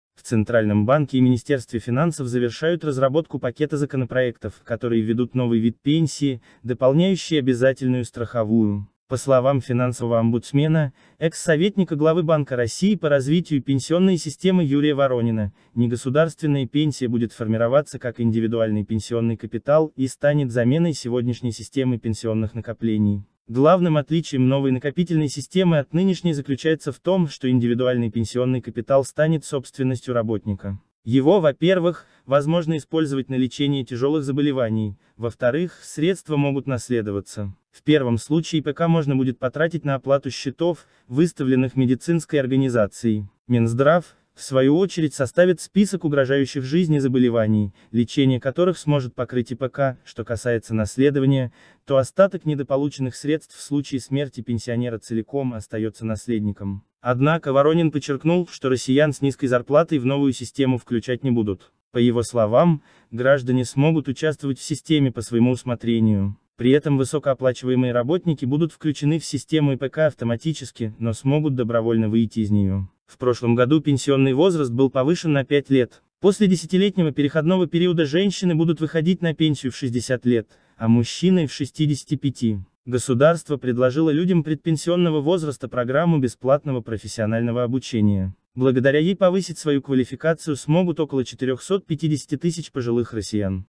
Новый вид пенсии в России уже скоро (ИПК)
По словам финансового омбудсмена, экс-советника главы Банка России по развитию пенсионной системы Юрия Воронина, негосударственная пенсия будет формироваться как индивидуальной пенсионный капитал (ИПК) и станет заменой сегодняшней системы пенсионных накоплений.